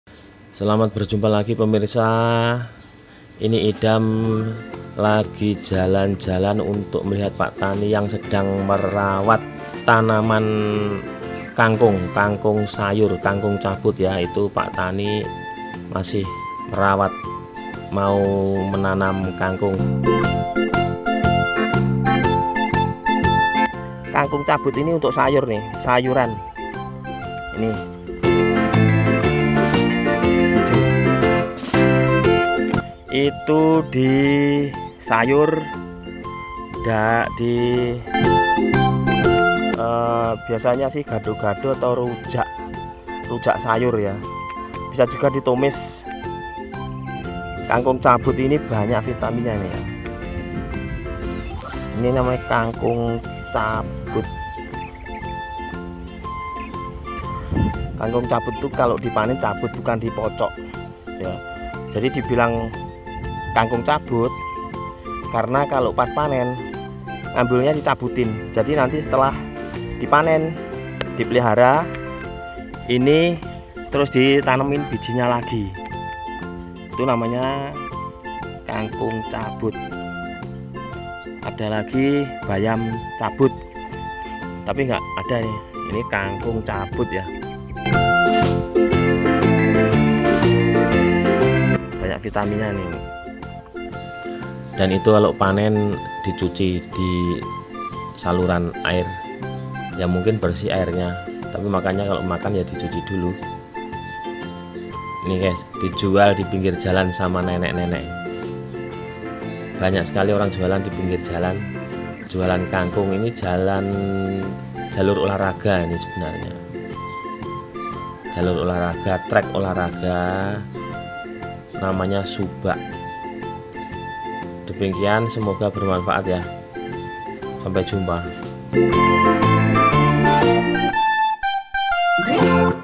DUA SISI -Menelusuri Ladang Kangkung Cabut
Selamat datang di idam co
Menelusuri Ladang Kangkung Cabut
kangkung adalah jenis tanaman yang hidup di air,kangkung cabut sayuran yang murah meriah untuk masyarkat dan banyak vitaminnya,kangkung bisa dimasak tumis maupun disayur sebagai pelengkap gado gado atau rujak cingur.kangkung yang sudah dimodifikasi sama petani ini sekali panen langsung ditanam bijinya lagi.dan sangat mudah perawatannya.
Membudidayakan kangkung cabut tidak membutuhkan perawatan intensif. Jika pemupukan sesuai takaran dan tepat waktu, maka kangkung cabut bisa dipanen saat usia 25 hari. Tapi hati-hati dari serangan belalang yang bisa merusak daun kangkung.

Selain menguntungkan, budidaya kangkung cabut ternyata juga tidak merepotkan. Dari benih hingga panen, kangkung cabut tak perlu ada perawatan yang intensif.

terima kasih jika menyukai video ini